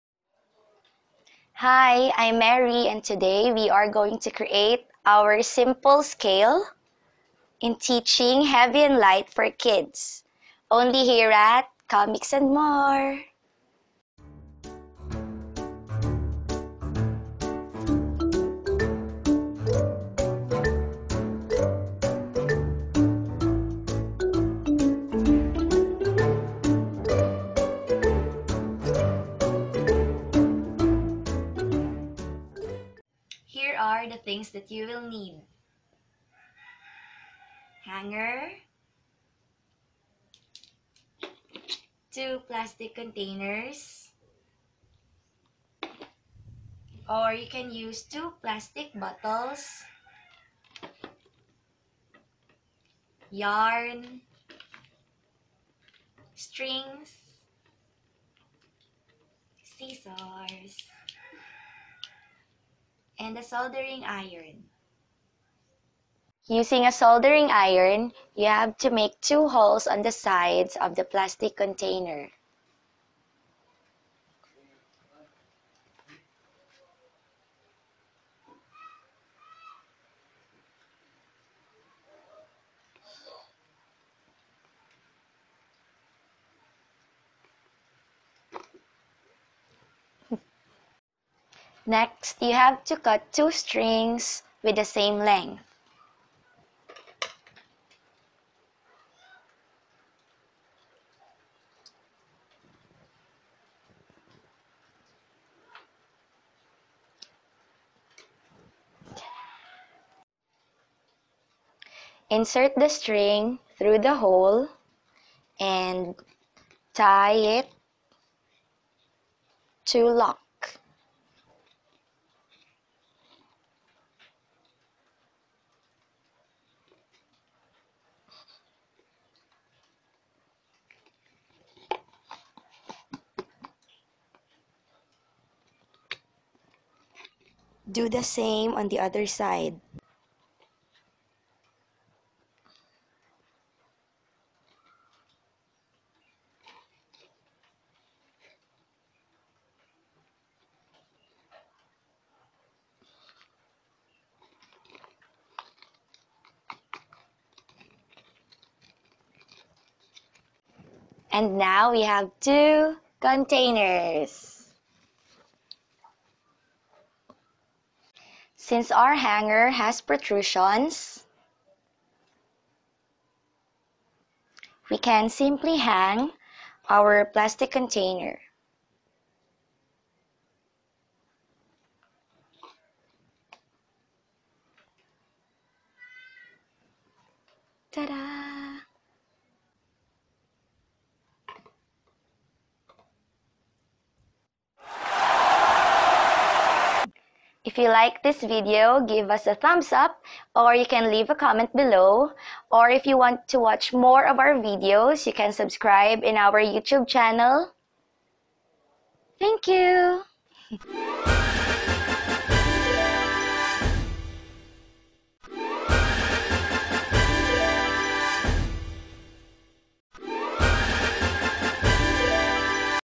How to Make a Simple Scale for Kids
Here in Commix and More we will demonstrate how to make your own simple scale for kids using materials you can find at home.

This instrument can be used in teaching the basic concept of weight (heavy and light) to kids.

The materials you need are the following:
a. Hanger
b. Plastic Containers or Bottles
c. Strings or Yarn
d. Scissors
e. Soldering Iron (or anything that can make a hole on the plastic container)

Steps:
1. Make holes on the plastic container.
2. Cut strings to equal lengths.
3. Tie the strings through the holes.
4. Hang the stringed containers on the hanger.